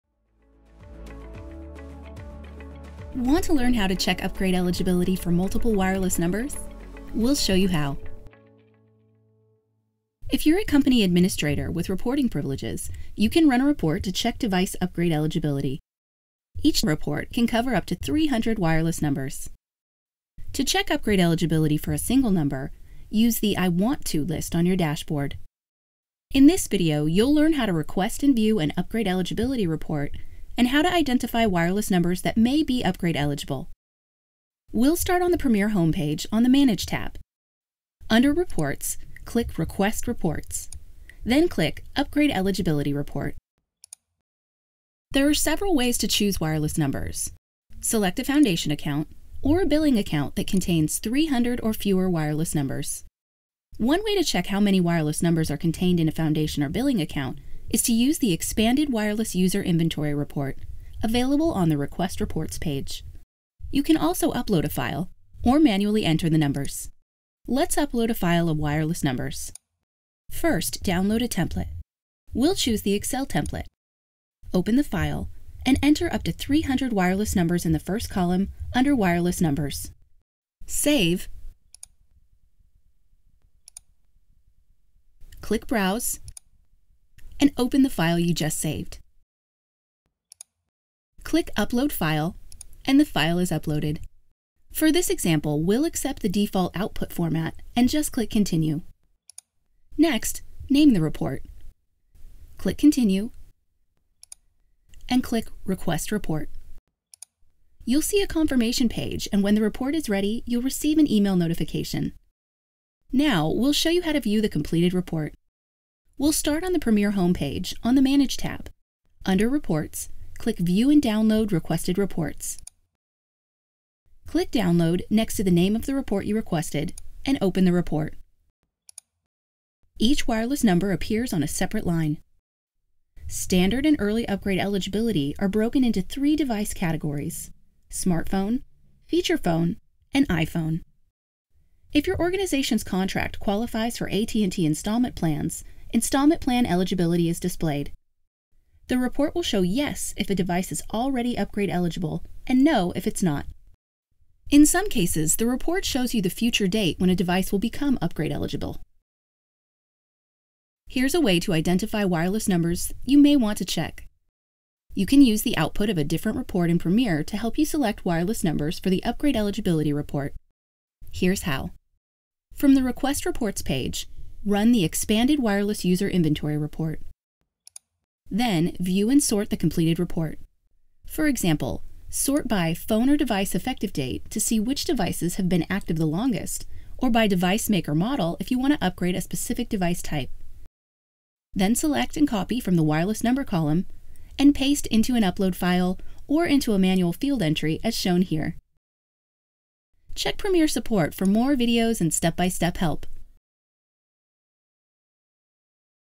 AT&T Premier: Check Upgrade Eligibility
If you’re a company administrator with reporting privileges, you can run a report to check device upgrade eligibility. Each report can cover up to 300 wireless numbers. In this video, we'll show you how.

Want to learn how to check upgrade eligibility for multiple wireless numbers?
We’ll show you how.
If you’re a company administrator with reporting privileges, you can run a report to check device upgrade eligibility. Each report can cover up to 300 wireless numbers.
To check upgrade eligibility for a single number, use the “I want to” list on your dashboard.
In this video, you’ll learn how to request and view an Upgrade Eligibility Report...
...and how to identify wireless numbers that may be upgrade-eligible.
We’ll start on the Premier home page, on the “MANAGE” tab.
Under “Reports,” click “Request Reports.”
And then click “Upgrade Eligibility Report.”
There are several ways to choose wireless numbers.
Select a foundation account …
…or a billing account that contains 300 or fewer wireless numbers.
One way to check how many wireless numbers are contained in a foundation or billing account is to use the “Expanded Wireless User Inventory Report,” available on the “Request Reports” page.
You can also upload a file…
...or manually enter the numbers.
Let’s upload a file of wireless numbers.
First, download a template.
We’ll choose the Excel template.
Open the file...
...and enter up to 300 wireless numbers in the first column, under “Wireless Numbers.”
Save,
click “Browse”,
and open the file you just saved.
Click “Upload File”…
…and the file is uploaded.
For this example, we’ll accept the default Output Format, and just click “Continue.”
Next, name the report…
…click “Continue”
…and click “Request Report.”
You’ll see a confirmation page and when the report is ready, you’ll receive an email notification.
Now we’ll show you how to view the completed report.
Under “Reports,” click “View and Download Requested Reports.”
Click “Download” next to the name of the report you requested…
…and open the report.
Each wireless number appears on a separate line.
Standard and early upgrade eligibility are broken into three device categories: Smartphone, Feature phone, and iPhone.
If your organization’s contract qualifies for AT&T installment plans, Installment Plan eligibility is displayed.
The report will show “Yes” if a device is already upgrade eligible, and “No” if it is not.
In some cases, the report shows you the future date when a device will become upgrade eligible.
Here’s a way to identify wireless numbers you may want to check.
You can use the output of a different report in Premier to help you select wireless numbers for the Upgrade Eligibility Report.”
Here’s how…
From the “Request Reports” page, run the “Expanded Wireless User Inventory Report.”
Then view and sort the completed report.
For example, sort by “Phone or Device Effective Date” to see which devices have been active the longest, or by Device Make or Model if you want to upgrade a specific device type.
Then select and copy from the “Wireless Number” column...
...and paste into an upload file or into a manual entry field, as shown here.
Check Premier Support for more videos and step-by-step help.